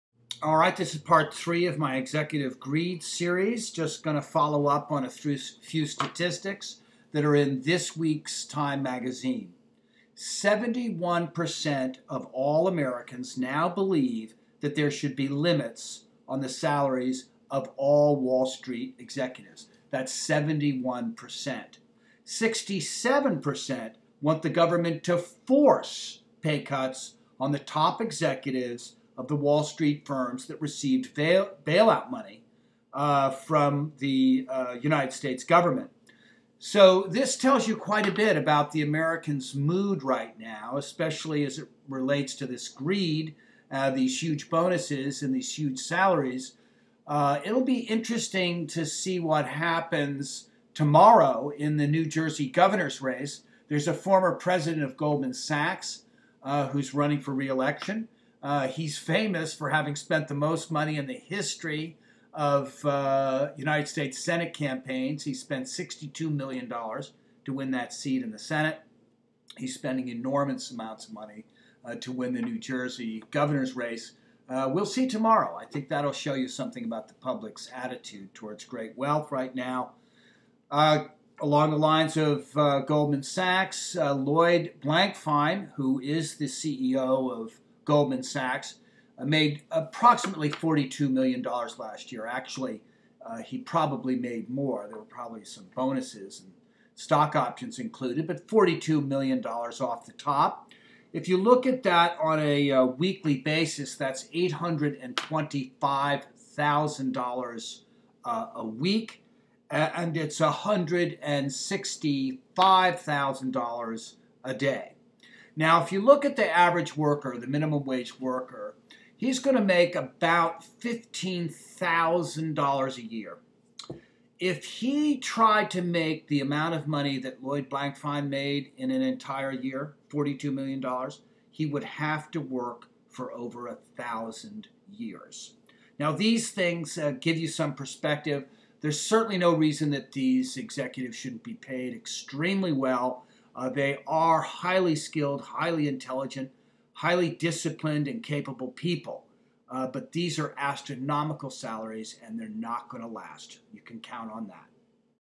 Executive Greed, Greedy, Greedier part 3
An examination of current trends of executive compensation on Wallstreet.